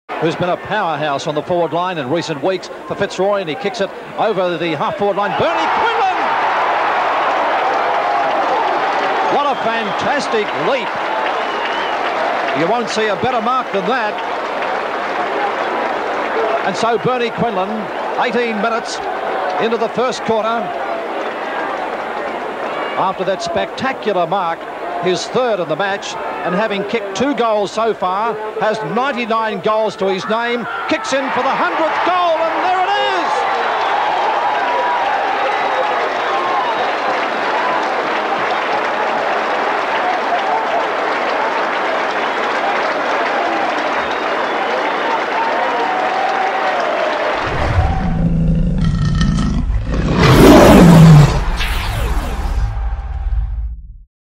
TBT: Bernie Quinlans' 100th Goal 1984
In this weeks Throwback Thursday we go back to 1984 when Fitzroys' Bernie Quinlan scored his 100th goal for the season